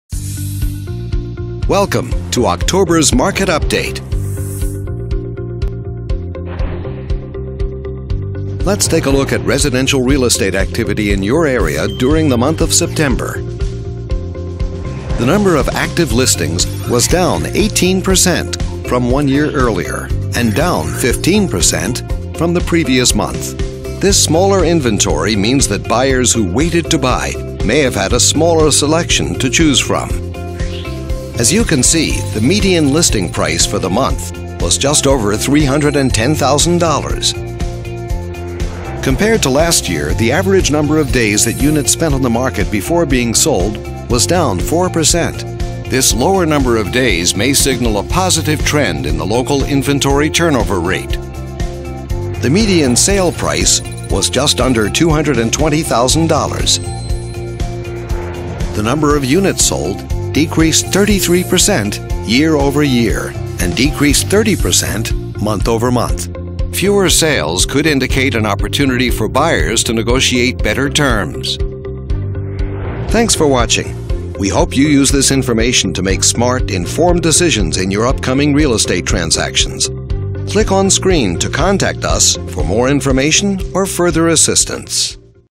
Mandarin Market Update,FL, Real Estate Market Update from Watson Realty,October, 2017
Watson Realty Video Update recapping market activity within Mandarin Market Update, FL, during the month of October, 2017